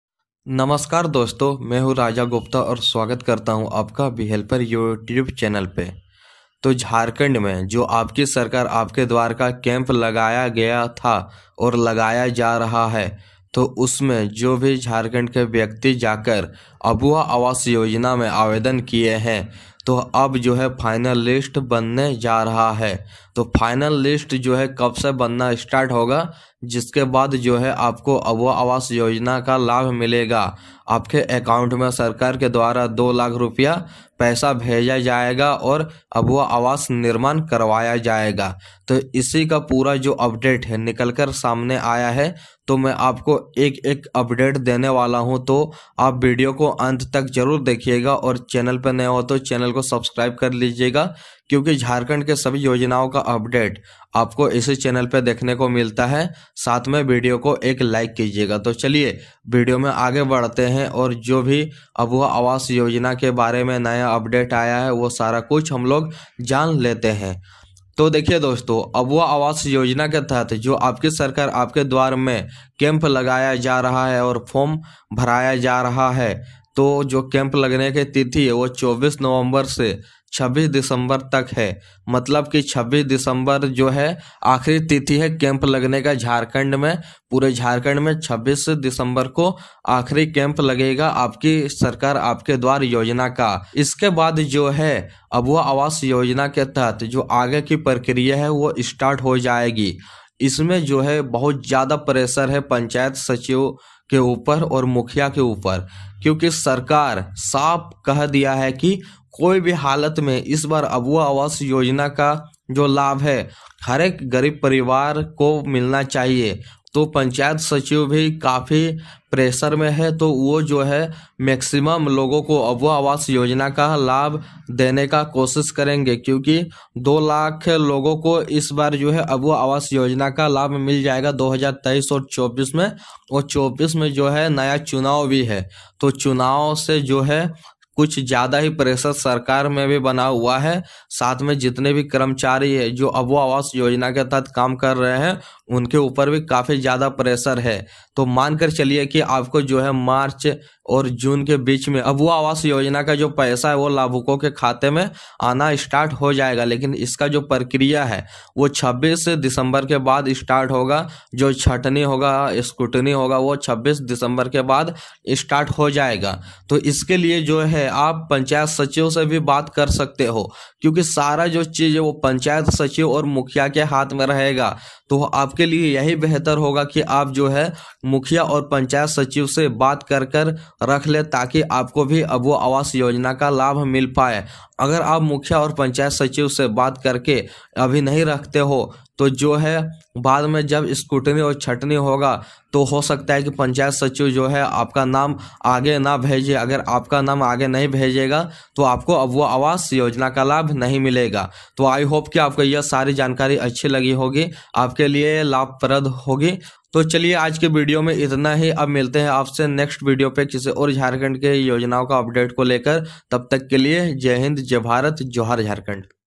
झारखण्ड अबूआ आवास योजना 2023 का पैसा इस प्रकार से आएगा
Jharkhand abua aawas Yojana ke tahat jitne log apply kiye Hain un sabhi ko abua aawas Yojana ka Paisa milega iske liye Sarkar ne Panchayat sachiv aur mukhiya per bahut jyada pressure banae hua hai Jaise hi Panchayat sachiv ke dwara fir se ek scootani aur chatne Kiya hua list aage bheja jaega uske bad sabhi logon Ko Paisa Milana start ho jaega ab vah aawas Yojana ka